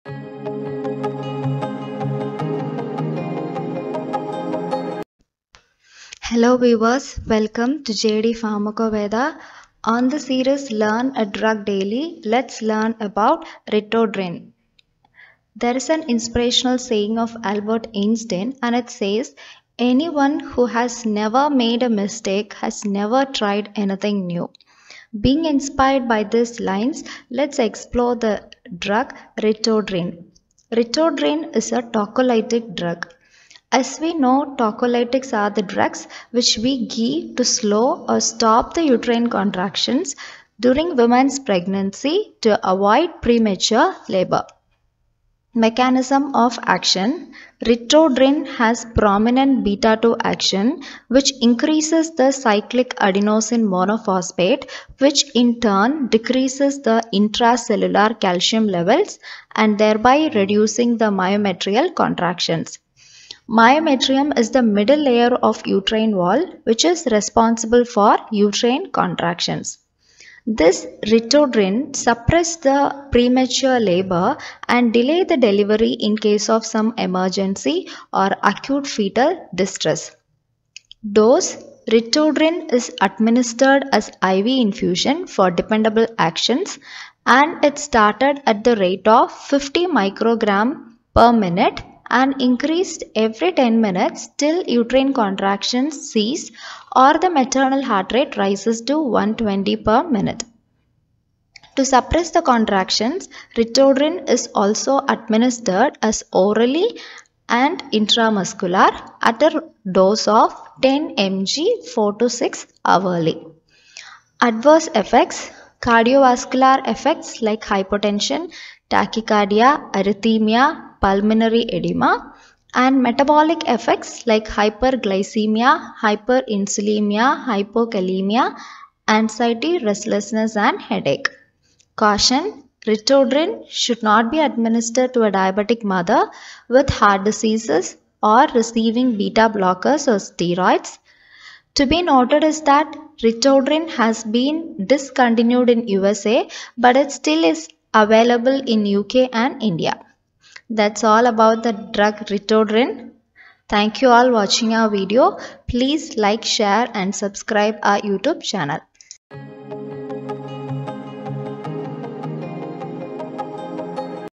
Ritodrine | Tocolytics | Pharmacology | GPAT | NIPER | MRB | EXIT EXAM | RAILWAY | ESIC | AIIMS
Ritodrine is a tocolytic drug (Tocolytics are medicines that are used to slow or stop the contractions of a woman's uterus during pregnancy) used to stop premature labor.

Mechanism of action: Prominent β2 action which increases cyclic AMP, which decreases intracellular calcium levels, thereby decreasing myometrial contractility (the myometrium is the middle layer of the uterine wall and is responsible for inducing uterine contractions).

Dose and actions:
It is approved to suppress premature labour and to delay delivery in case of some exigency or acute foetal distress.
* For dependable action it is started as
50 µg/min i.v. infusion, the rate is increased every 10 min till uterine contractions cease or maternal HR rises to 120/min.
* Contractions are kept suppressed by continuing i.v. infusion or by 10 mg i.m. 4–6 hourly followed by 10 mg oral 4–6 hourly.

Adverse effects: cardiovascular effects ( hypotension, tachycardia, arrhythmia, pulmonary edema) and metabolic effects like hyperglycaemia, hyperinsulinaemia, hypokalaemia and anxiety, restlessness, headache occur frequently.

Caution:
It should not be used if mother is diabetic, having heart disease, or receiving β blockers or steroids.
Ritodrine has been discontinued in the USA, but is still available in UK and India.